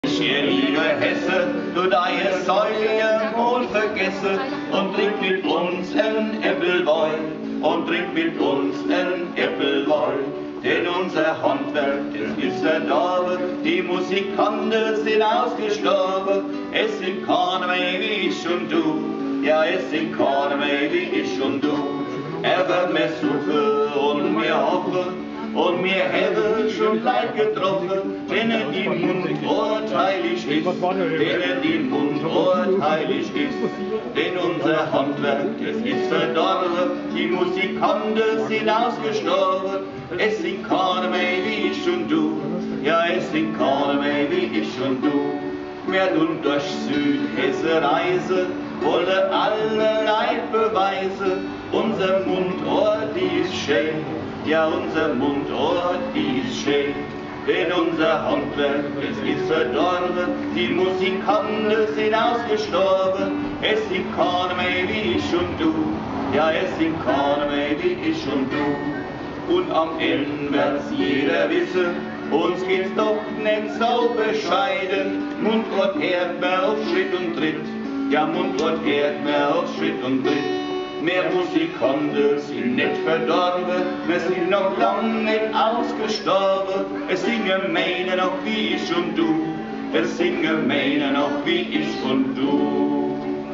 Der Odenwälder Guggugg auf Wandertournee
Der Odenwälder Guggugg, Jürgen Poth und Fritz Ehmke lassen sich auch bei Regenwetter nicht von Ihren Wanderungen abhalten. Heute sind die beiden vom Marktplatz in Michelstadt auf dem EM1 nach Erbach gestartet, wo es am Abend im Erbacher Brauhaus eine Veranstaltung mit Musik und Mundart gibt. Vor dem Start gab es noch ein Lied. Hoffentlich hält die Stimme unseres Guggug bei dem naßkalten Wetter durch. Wir freuen uns schön, wenn er am 14.06. um 18 Uhr die Odenwälder Lammwochen mit eröffnet.
Wer den tollen Panoramaweg Erbach-Michelstadt nachwandern möchte, der findet eine Karte und Wegbeschreibung